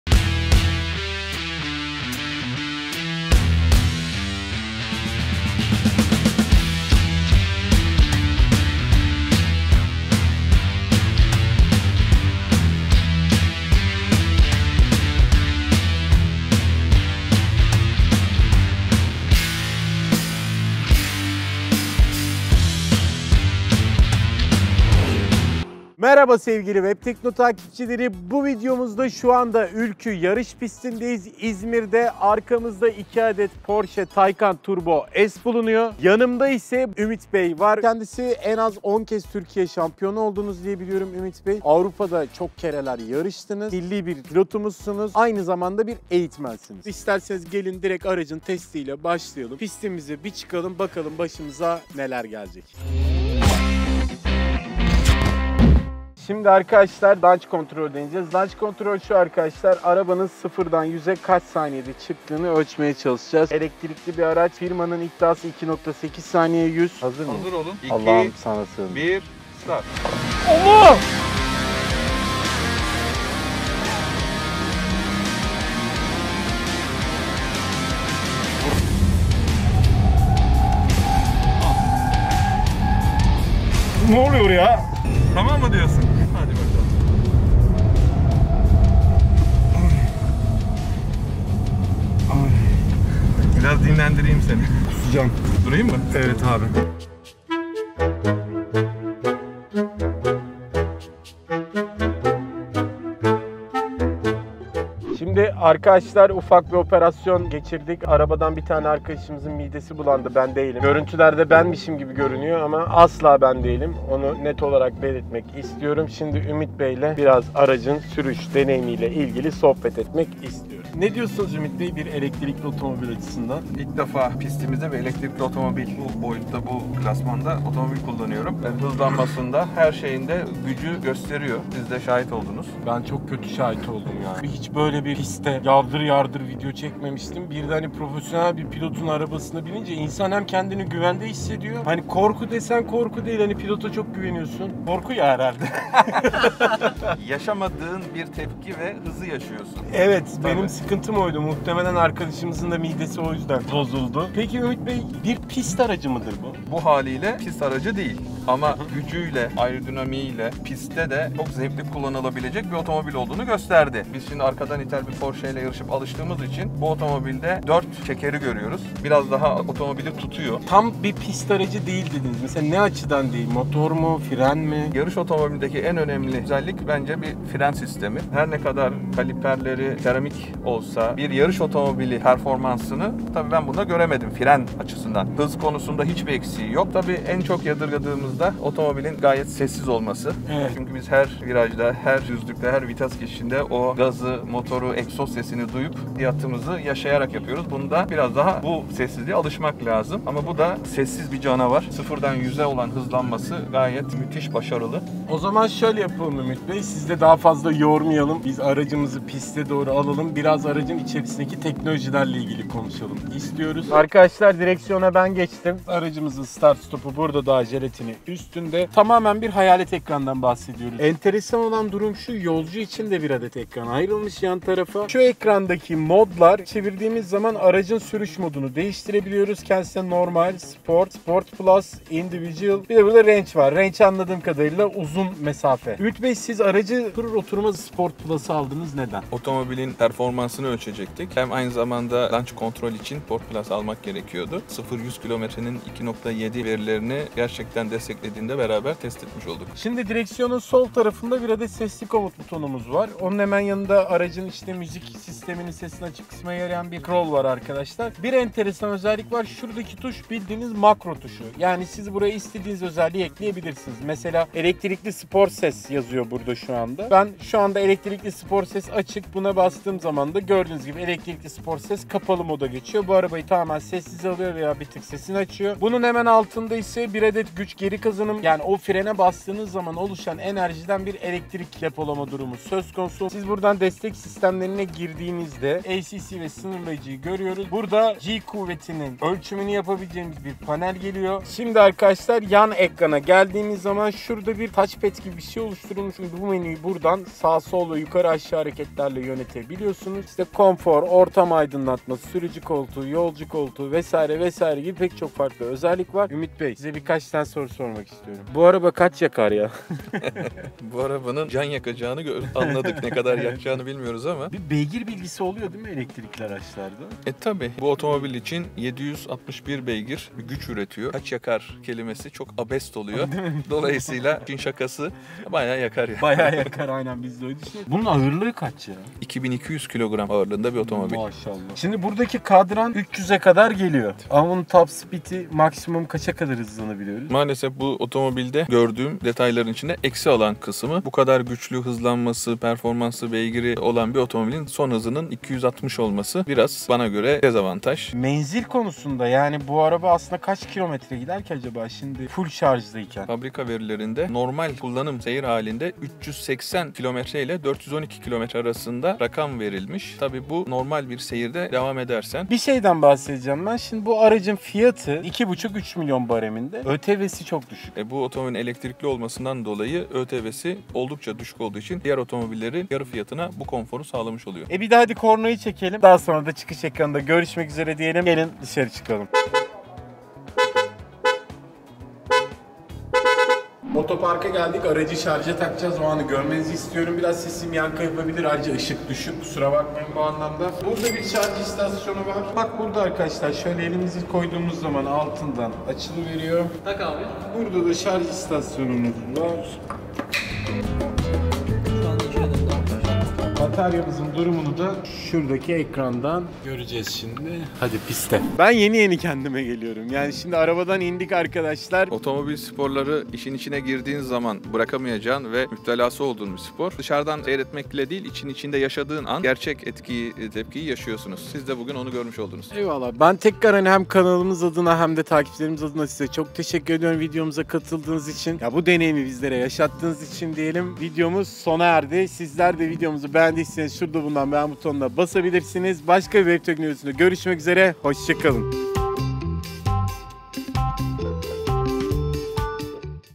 Elektrikli Roket Porsche Taycan Turbo S’i Pistte Denedik!
Son yılların en çok ses getiren arabalarından Porsche Taycan Turbo S’i yarış pistinde test ettik!

Launch Control özelliği sayesinde 0’dan 100’e 2,8 saniyede çıkabilen Porsche Taycan Turbo S, tam 761 beygir gücünde. Saatte maksimum 260 kilometre hıza çıkan Taycan’da 560 kW gücünde bir batarya bulunuyor. Her 100 kilometrede 28,5 kW enerji harcıyor. Benzinli olmadığı için de doğal olarak karbon salınımı yapmıyor.
İki adet elektrikli motoru bulunan Taycan Turbo S’in dört tekeri de bu motorlara bağlı. Bu sayede kalkış, sürüş ve yavaşlama esnasında, rakibi benzinli spor otomobillere kıyasla çok daha çevik hareketler yapabiliyor. Videoda da görebileceğiniz üzere, pistteki kalkış anında içindeki yolculara Newton’un birinci yasası olan eylemsizliği tam anlamı ile yaşatıyor.
Trafiğe kapalı alanda test ettiğimiz Porsche ‘Taycan’ Turbo S’in adı eski Türkçeden geliyor. Anlamı ‘güçlü ve genç bir atın ruhu’ olan Taycan, test ettiğimiz Turbo S versiyonu ile elektrikli spor otomobil kategorisinde Tesla gibi devlere de meydan okuyor. Porsche Taycan Turbo S’in fiyatı, özellikleri ve diğer detaylar ise videoda…